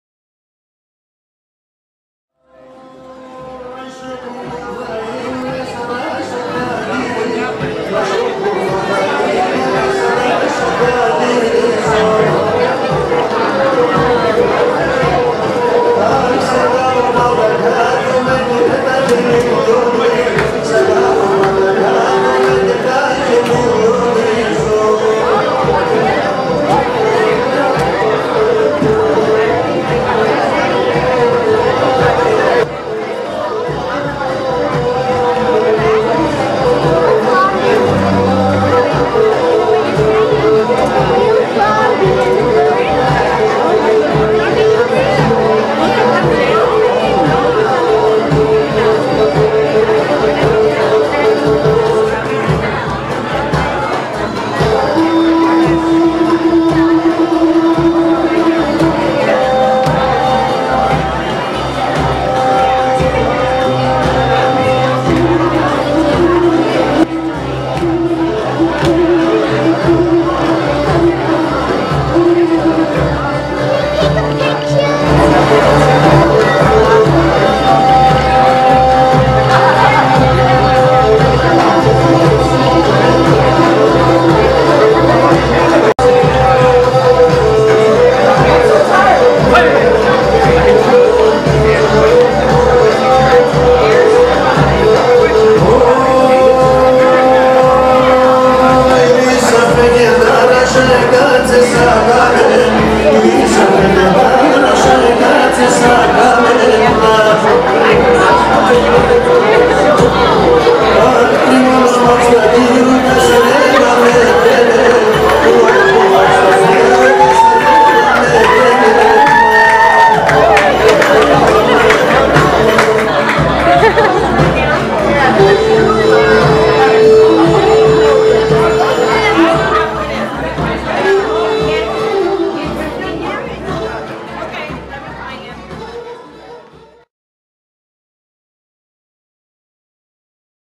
Christmas Dancing st George 2015
Greek Dancing at the St. George Christmas Party